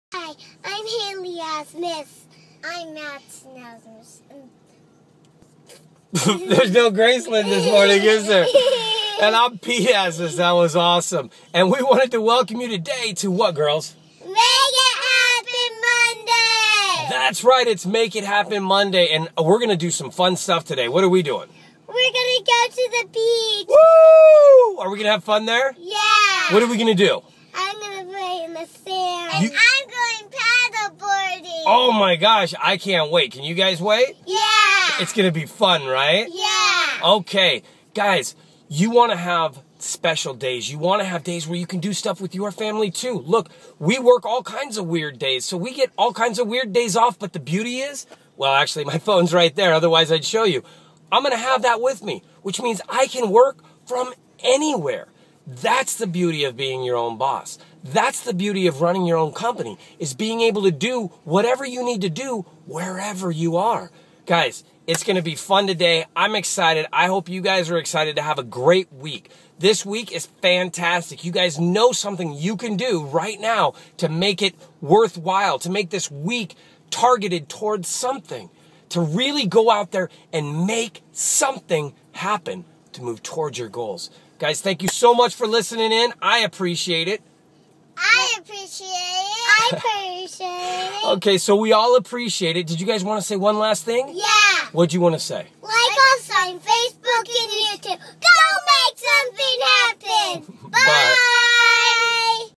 Pete Asmus- Make it happen Monday!
Pete Asmus talks about what they are doing today and how to make the most out of your life! Super funny today too!
Transcription..

Hailey: Hi, I'm Hailey Asmus.
Madisen: I'm Madisen Asmus. And this...
Pete: What? There's still Graicelynn this morning, is there? And I'm Pete Asmus! That was awesome! And we wanted to welcome you today to what, girls?
Madisen and Hailey: MAKE IT HAPPEN MONDAY!
Pete: That's right! It's Make It Happen Moday! And we're gonna do some fun stuff today. What are we doing?
Hailey: We're gonna go to the beach!
Pete: Whoo! Are we gonna have fun there?
Madisen and Hailey: Yeah!
Pete: What are we gonna do?
Hailey: I'm gonna play in the sand!
Madisen: And I'm going paddle boarding!
Pete: Oh my gosh! I can't wait! Can you guys wait?
Pete: It's gonna be fun, right?
Pete: Okay! Guys, you want to have special days. You wanna have days to do stuff with your family too! Look, we work all kinds of weird days! We get all kinds of weird days off! But the beauty is—well, actually my phone's right there, so I can't show you. I'm gonna have that with me, which means I can work from anywhere. That's the beauty of being your own boss. That's the beauty of running your own company. Is being able to do what you're able to do wherever you are. Guys, it's gonna be fun today! I'm excited! I hope you guys are excited to have a great week! This week is fantastic. You guys know something you can do right now to make it worthwhile, to make this week targeted towards something, to really go out there and make something happen to move towards your goals. Guys, thank you so much for listening! I appreciate it!
Madisen: I appreciate it!
Hailey: I appreciate it!
Pete: Okay, so we all appreciate it! You guys wanna say one last thing?
Pete: What do you guys wanna say?